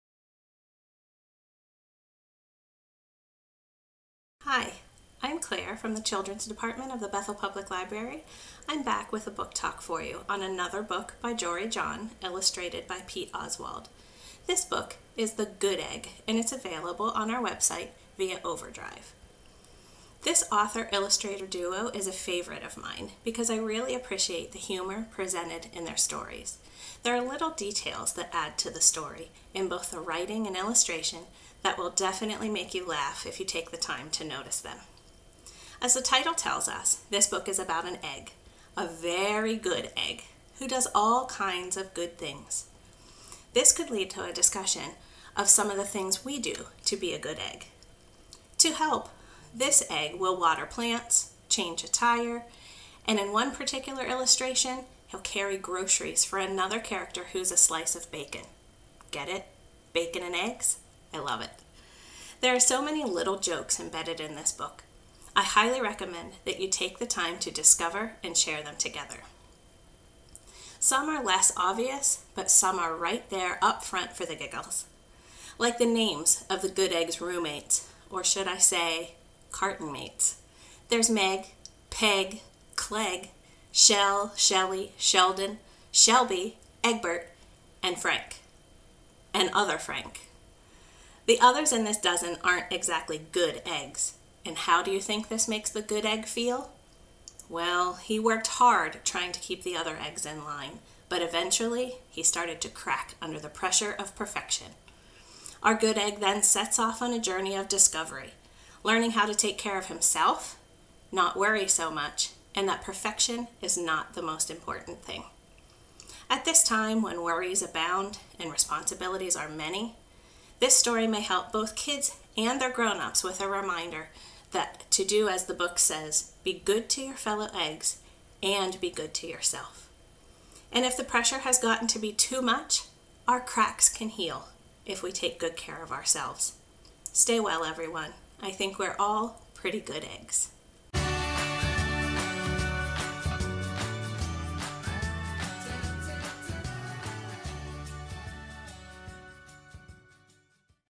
Bethel Book Talks: The Good Egg by Jory John
Listen to a story about not having to be Grade A perfect all the time!